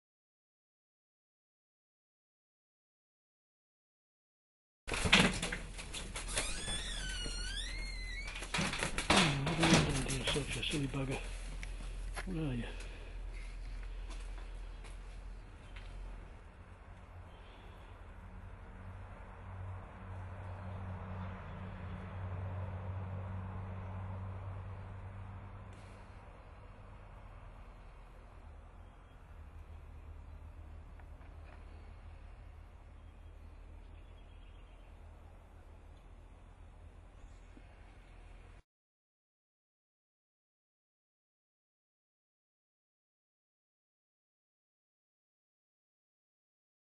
RATCATCHER SUPREME
We call them chickenhawks here but there is likely a correct name which I have not bothered thus far to find out. This one came in on the back verander after some rats or bandicoots and became blinded by the extreme backlight and couldn't find its way out the door again. After taking this quick image, I opened the far door which is shaded, closed the door nearest and the bird flew out without furhtur fuss.

It was fairly weary as it had been tghere a while. I had been away about three hours churning up the bush with a big mower for the fire season. You will observe, the bird lets the right talons come off the mesh, ready armed for letting go with the other, rolling out and cleaning my clock with a set of tramtracks across the dial if I got too close and uppity.